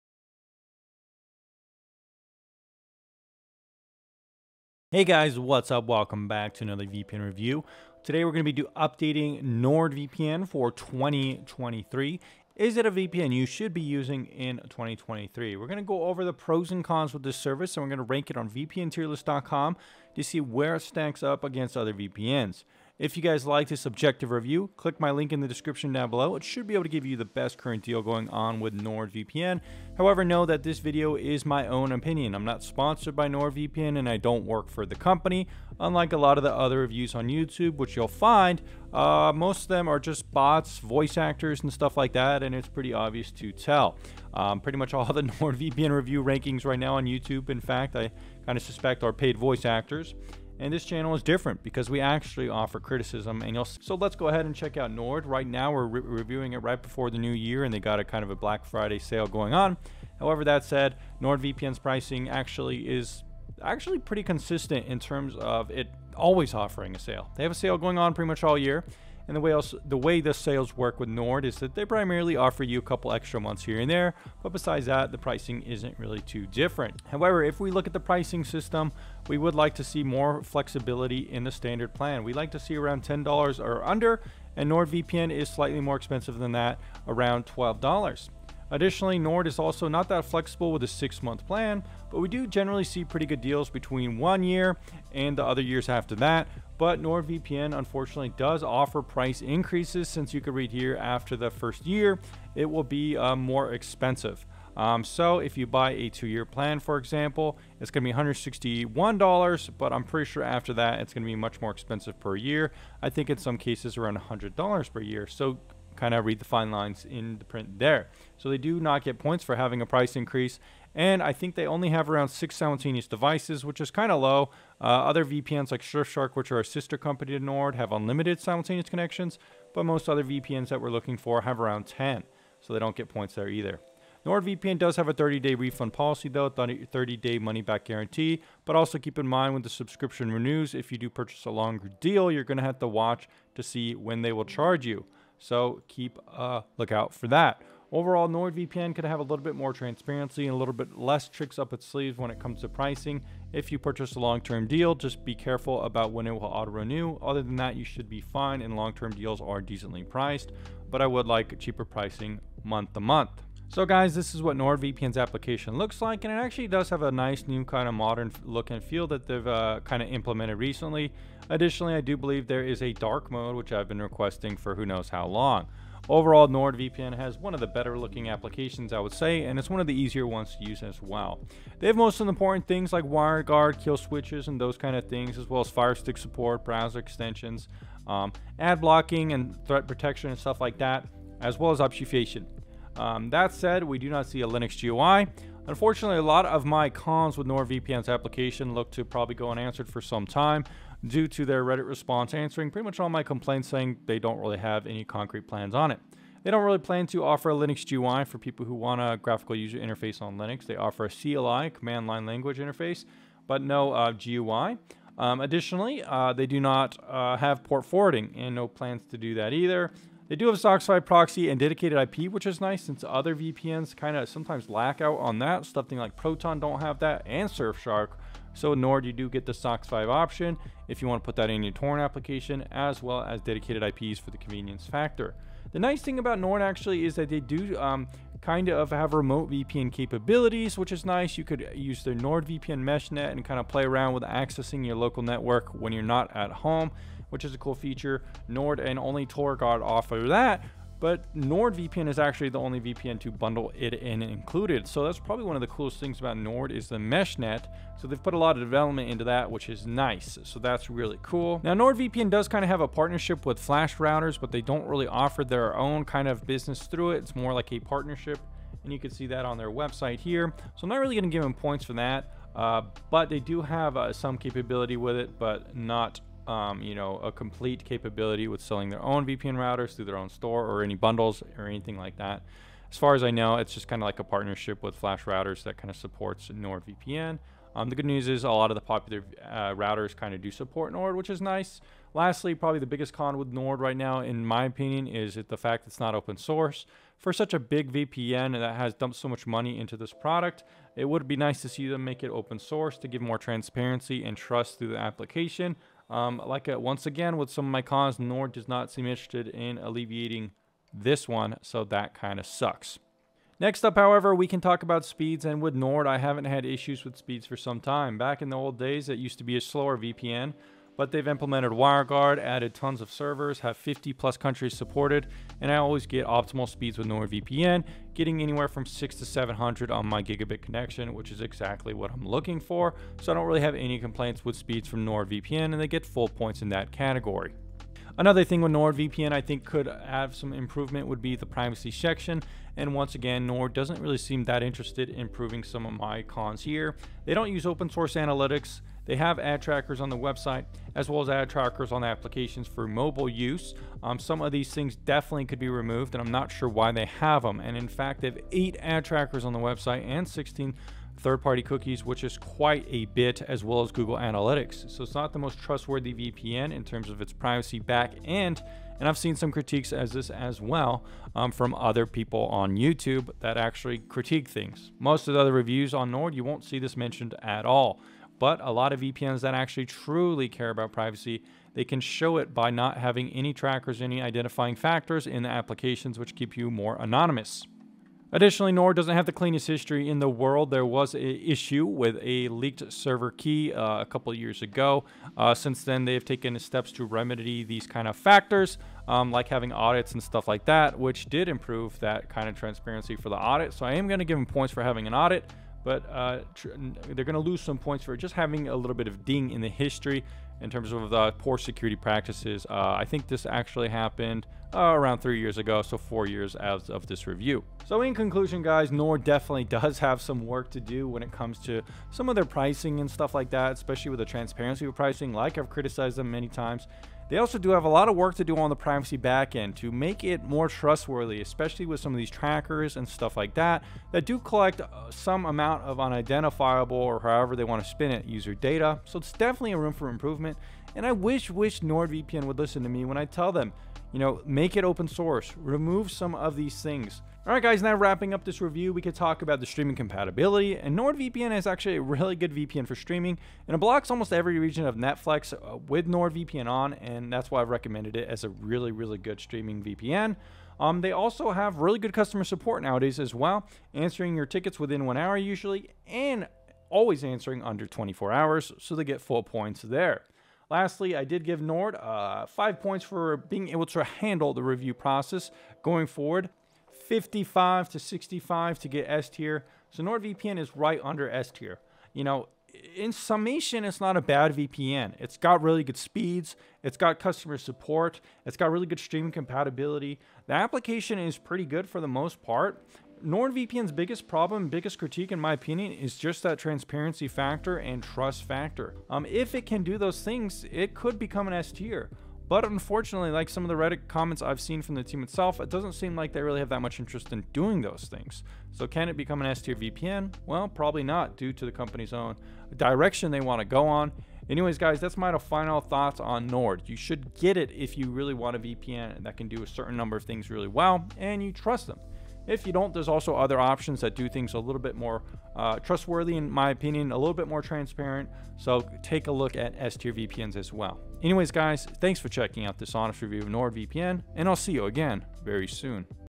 NordVPN Review 2023 : Brutally Honest Opinion
Here is my NordVPN review for 2023. Unlike most others on Youtube, this one is not sponsored and it's objective.
This is my honest and objective NordVPN review for 2023.

code "tomspark" may give you a discount

Timestamps:
00:00 Introduction
01:57 Pricing discussion
03:40 Application discussion
06:15 Speed discussion
06:37 Privacy discussion
09:03 Final Scores / Conclusion